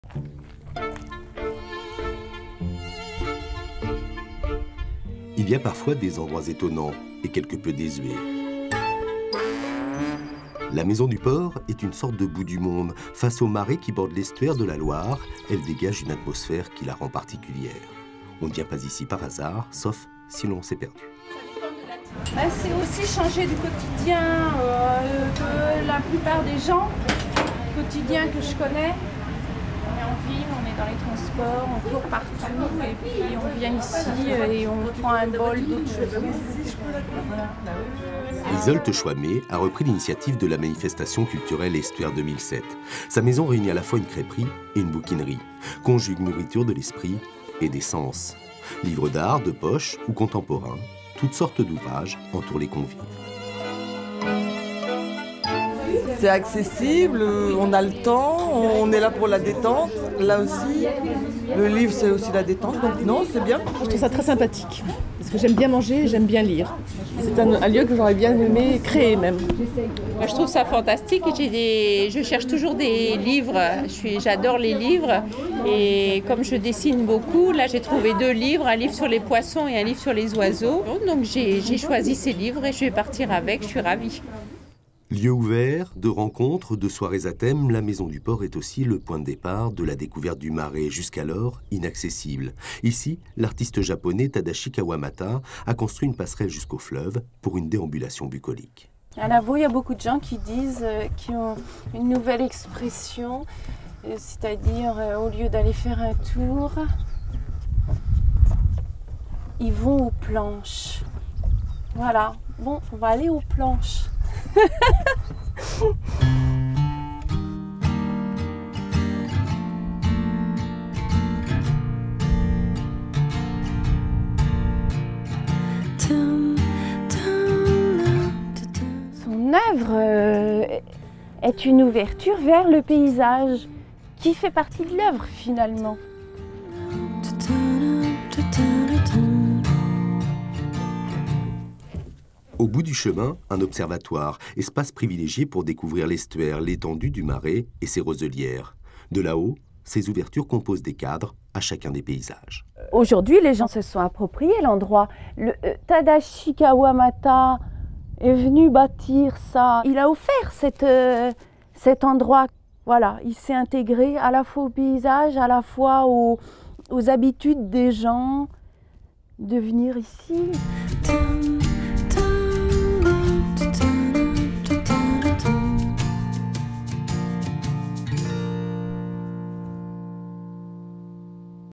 reportage : crêperie Bouquinerie à Lavau-sur-Loire
Partons à la découverte d’un village surprenant, Lavau sur Loire… Là-bas se trouve une maison qui fait à la fois crêperie et bouquinerie, introduction à une balade dans le marais où l’œuvre d’un artiste japonais a permis de redécouvrir les lieux…
Réalisation: Philippe Tanger
Intervenante : Yseult Choimet, restauratrice-bouquiniste
Date de diffusion : 19 juin 2011